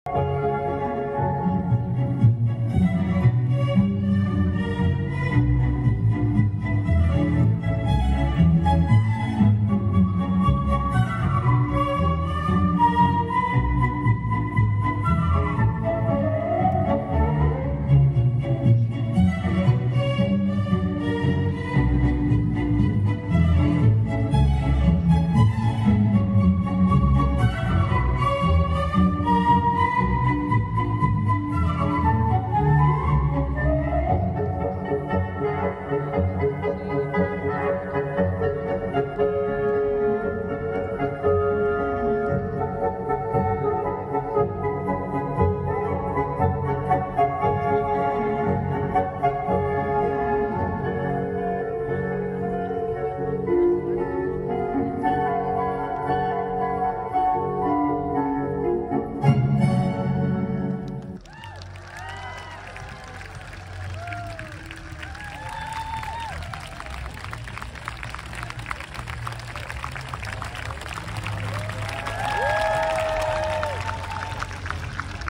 Vietnam National Water Puppet Theatre - Global Village - Sziget 2022
Vietnam National Water Puppet Theatre at the Global Village - Sziget 2022 – Friday 12th 2022